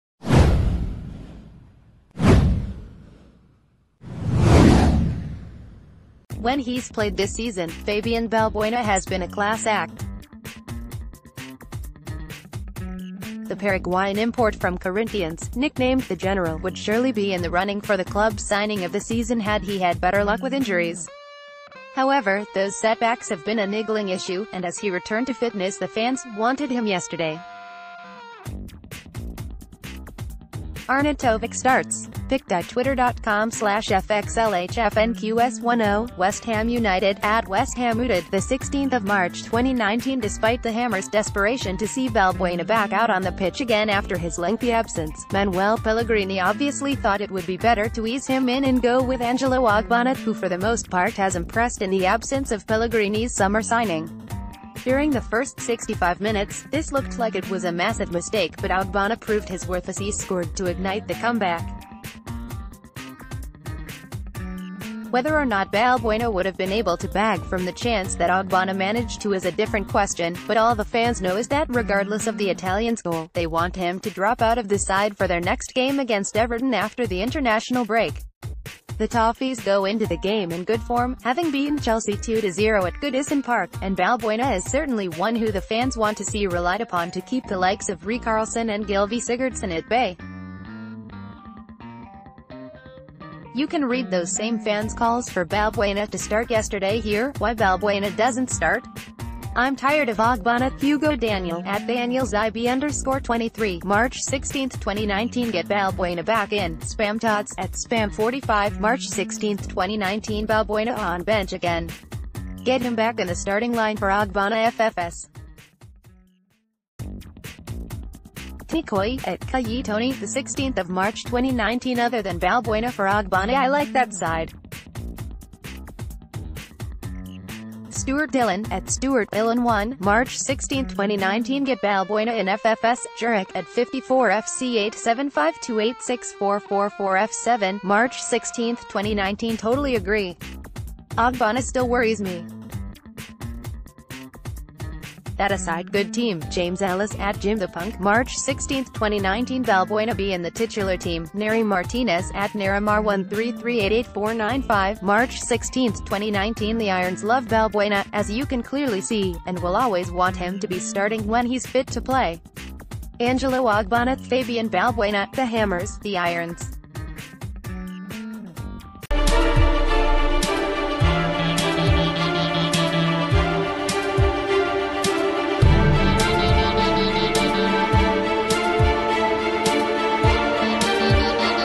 West Ham fans took to Twitter in a rage when seeing Balbuena benched
When he’s played this season, Fabian Balbuena has been a class act. The Paraguayan import from Corinthians, nicknamed ‘The General’ would surely be in the running for the club’s signing of the season had he had better luck with injuries. However, those setbacks have been a niggling issue, and as he returned to fitness the fans wanted him yesterday. Arnautovic starts! — West Ham United (@WestHamUtd) 16 March 2019 Despite the Hammer’s desperation to see Balbuena back out...